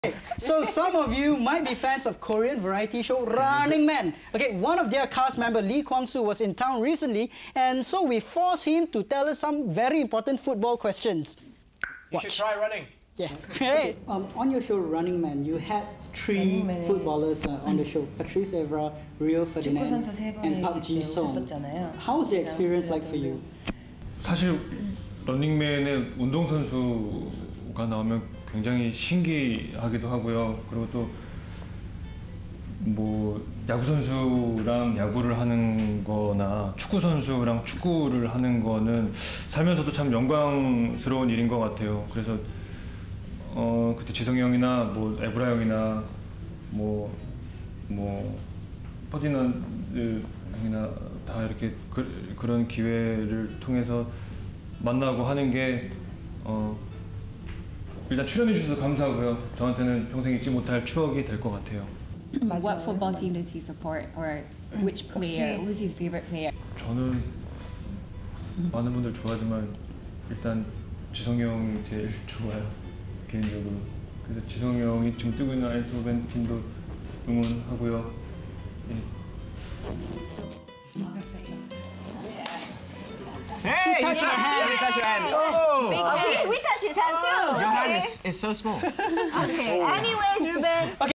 S4 E22: Who is Lee Kwang Soo's favourite footballer?
Lee Kwang Soo of Running Man gets grilled by Reem, Michelle and Reuben!